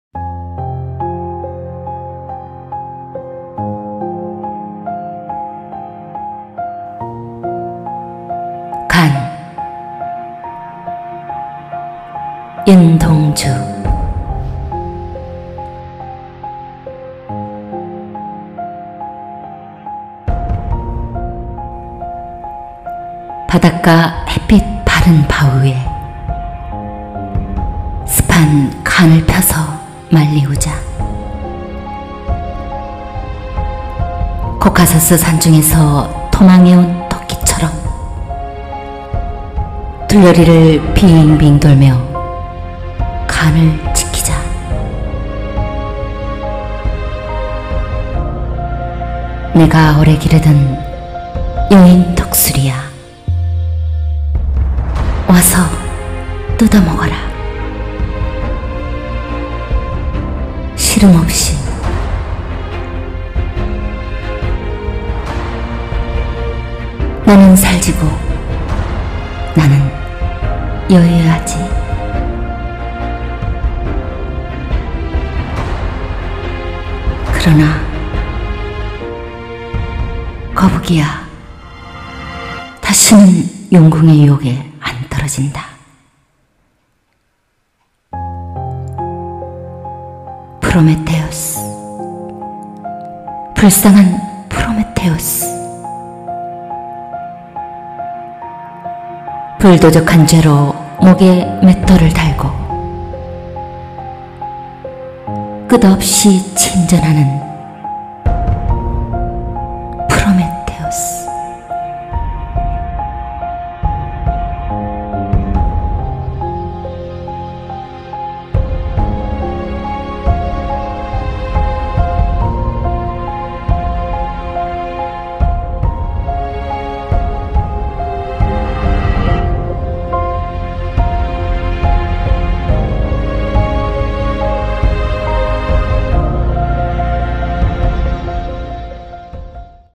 간-윤동주
바닷가 햇빛 바른 바위 우에
습한 간을 펴서 말리우자.

코카서스 산중에서 도망해 온 토끼처럼
둘러리를 빙빙 돌며 간을 지키자.

내가 오래 기르든 여윈 독수리야!
와서 뜯어먹어라. 시름없이

너는 살지고
나는 여위어야지. 그러나

거북이야
다시는 용궁의 유혹에 안 떨어진다.

프로메테우스 불쌍한 프로메테우스
불 도적한 죄로 목에 맷돌을 달고

끝없이 침전하는 프로메테우스